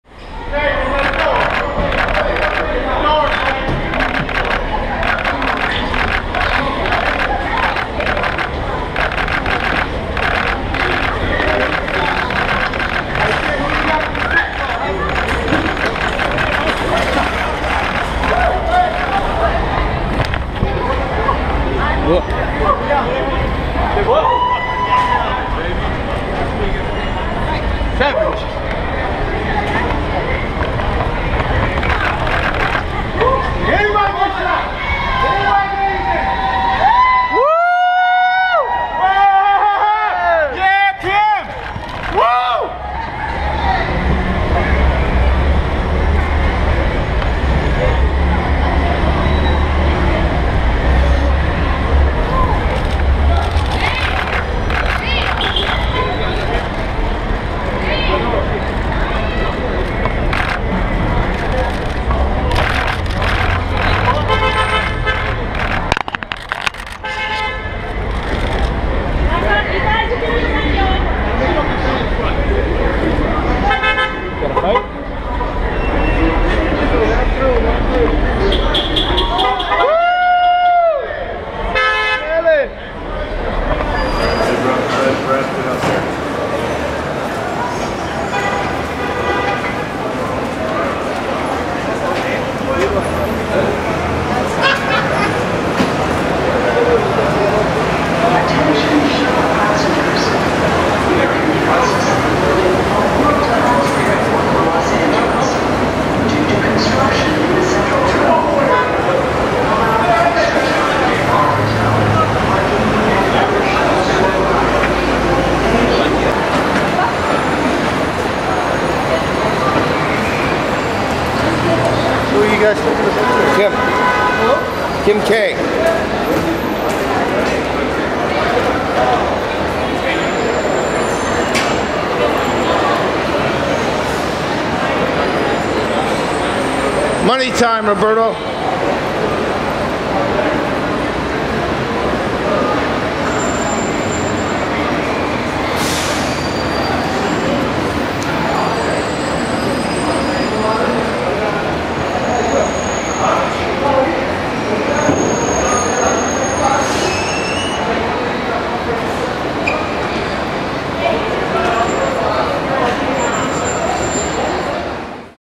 Kim Kardashian Caught In Photographer Chaos [2013]
Wednesday, October 2, 2013: A blonde Kim Kardashian returned to Los Angeles through LAX and was greeted by a horde of celebrity photographers.  X17online.com